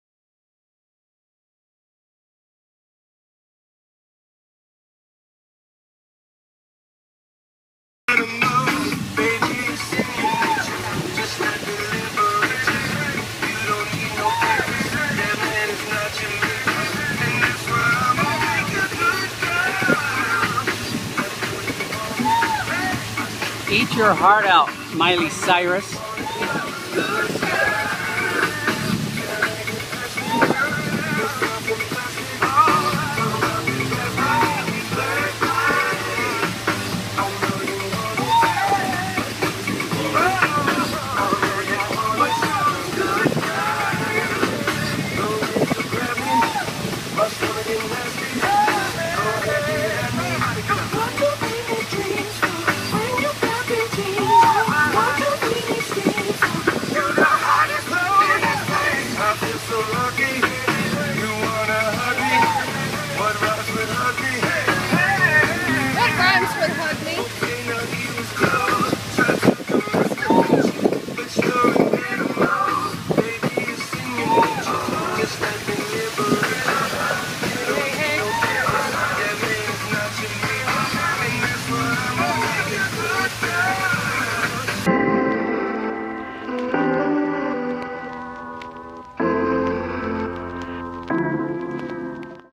Boat Dancing on the Sacramento River
We are boating on the Delta AKA the Sacramento River, while dancing and having fun we encountered a giant ship.